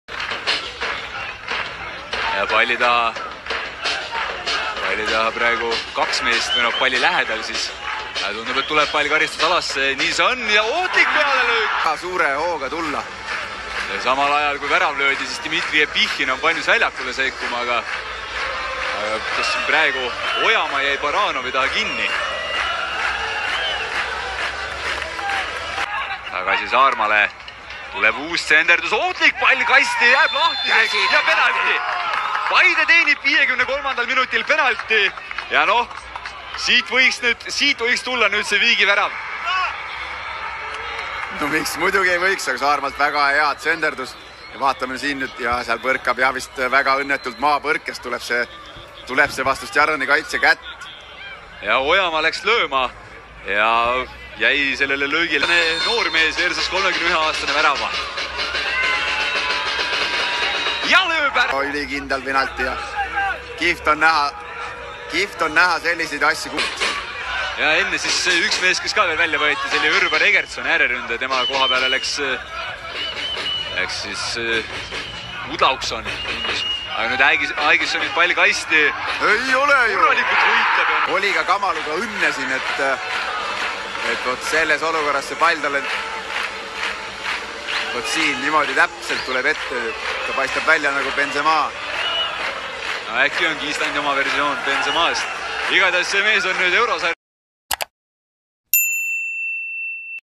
Stjarnan 2-1 Paide | CONFERENCE LEAGUE 2024/25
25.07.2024 Second qualifying round - 1st leg

Stjörnuvöllur - Gardabaer